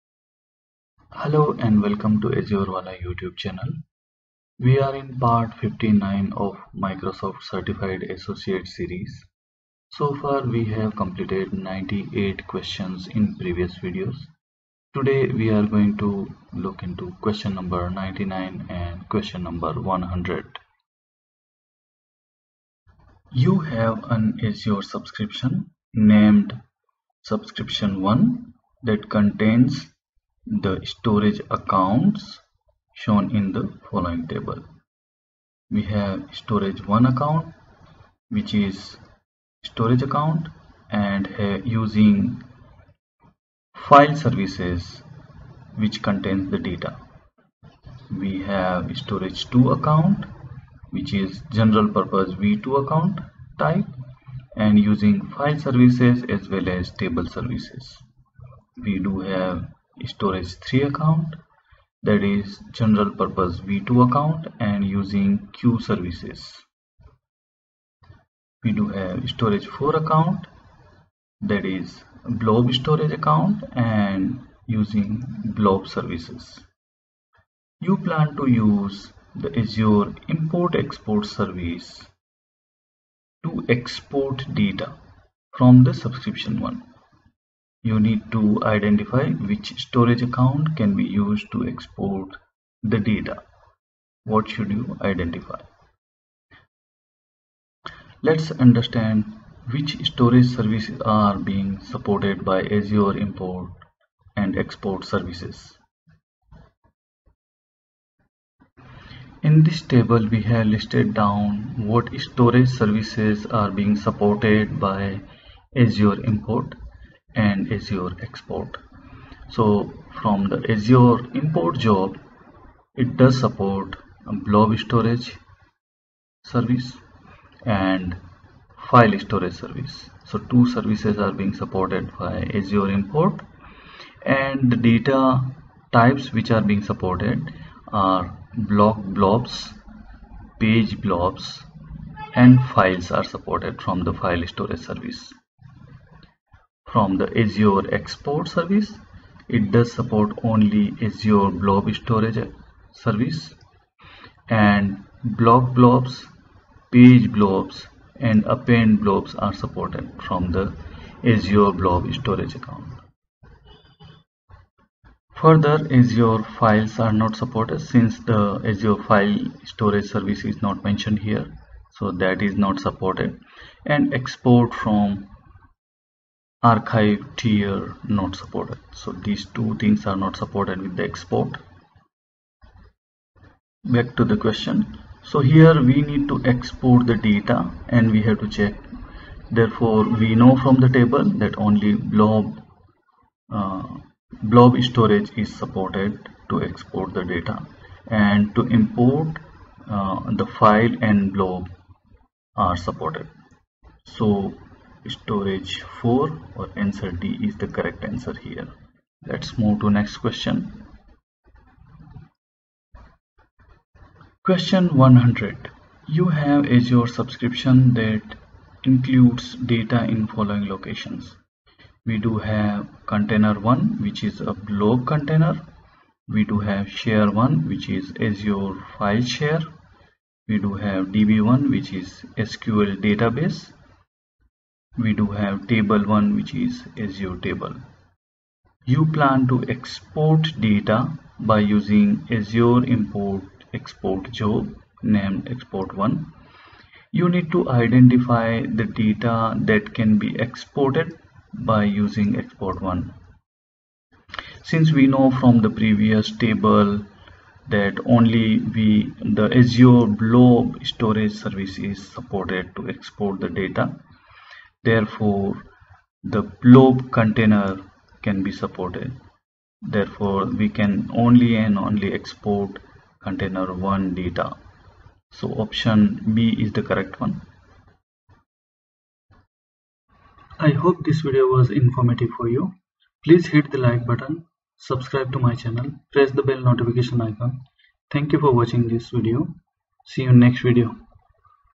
59. MS Azure Administrator Associate AZ 104 - Azure Import/Export Service, Blob / file storage
- Azure Blob storage
- Azure File storage
- Azure Import/Export